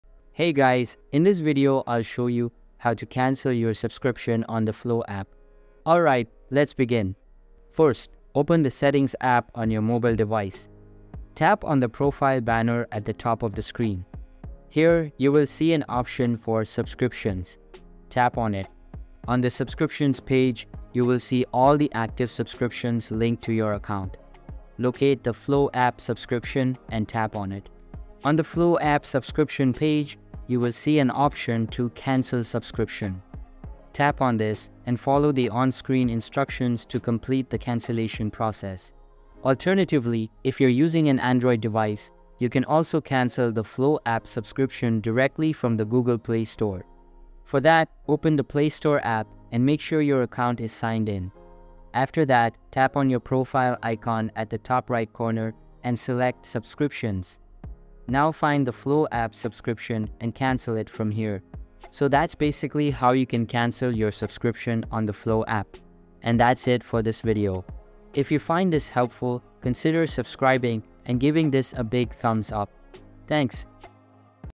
How To Cancel Subscription on Flo App | Unsubscribe From Flo App
Want to cancel your subscription on the Flo app? Follow this guide to easily unsubscribe from the Flo app!